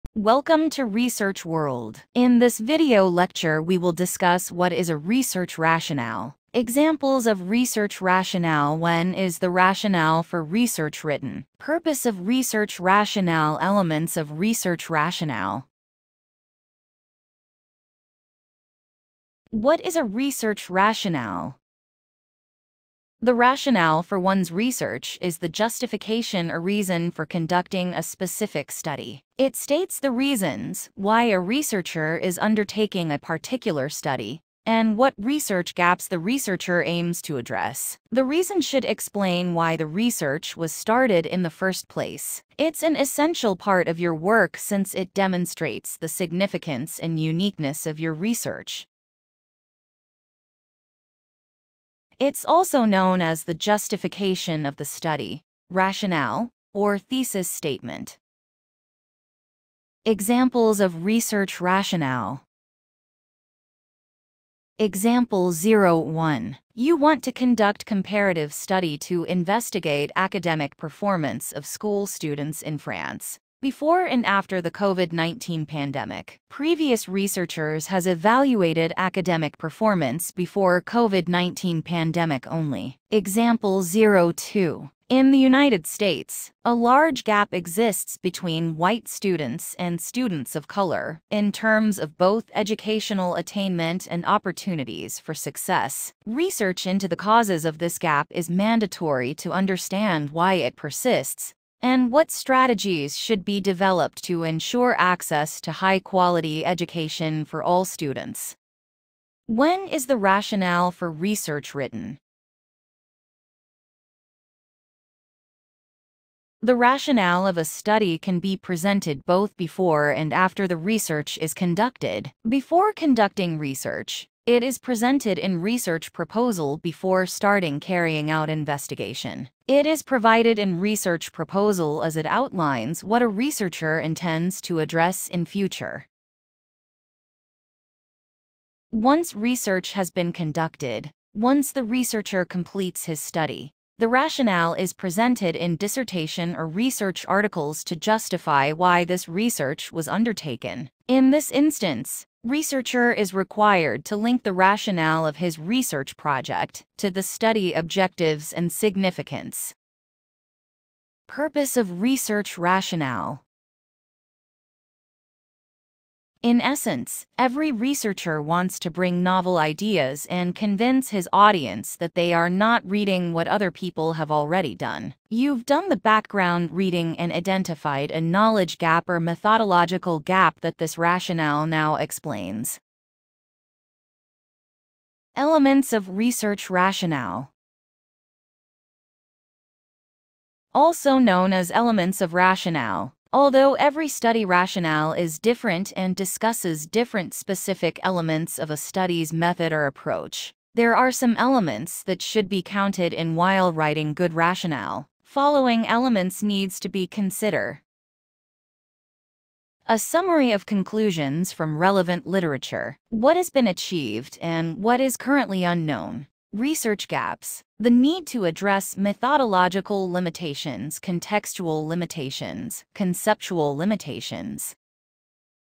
What is research rationale || How to write a research rationale || Rationale of the Study
The rationale for one’s research is the Justification or Reason for conducting a specific study. It states the reason(s) why a researcher is undertaking a particular study and what Research gaps the researcher aims to address.